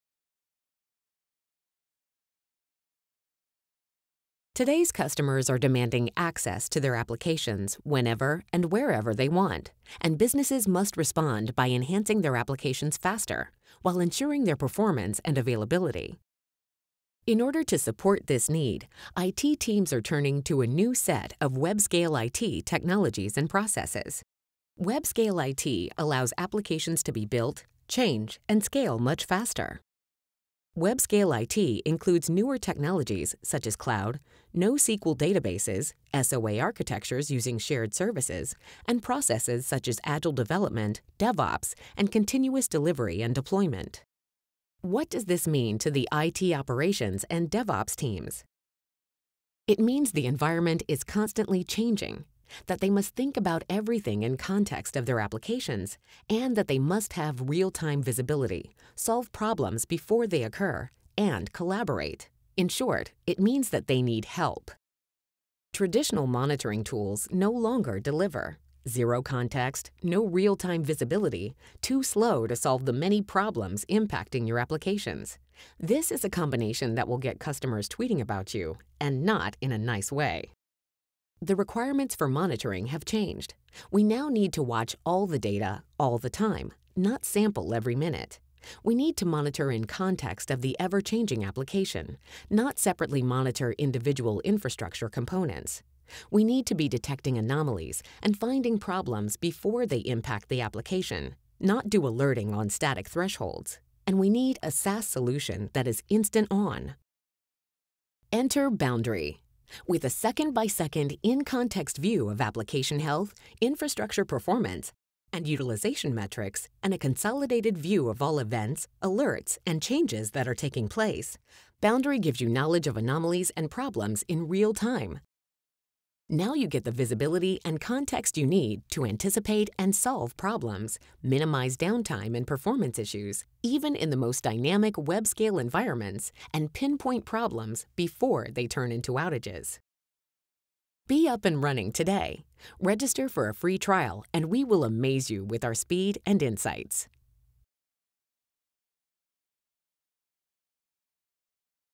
Boundary - Unified Monitoring for Web-Scale IT - Every Second Counts
The world of applications has changed with the adoption of Web-Scale IT and DevOps technologies and processes. Learn why current monitoring solutions don't work, and what to do about it.